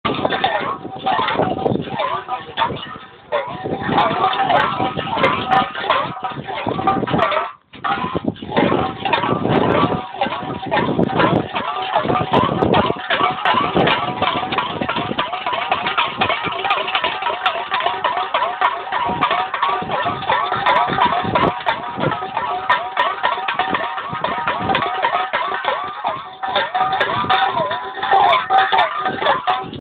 Rullande burken
En burk rullar med vinden längs med torget i Eslöv. Ett underbart ljud kommer ur den halvfulla behållaren.